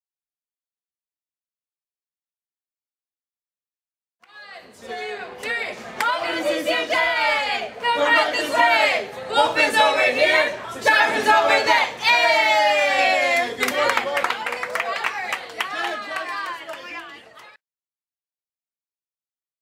TCNJ incoming freshmen receive enthusiastic welcome
EWING - Thursday, August 25, 2016Incoming freshmen get an enthusiastic welcome at The College of New Jersey freshman move-in day.(Michael Mancuso | For NJ.com)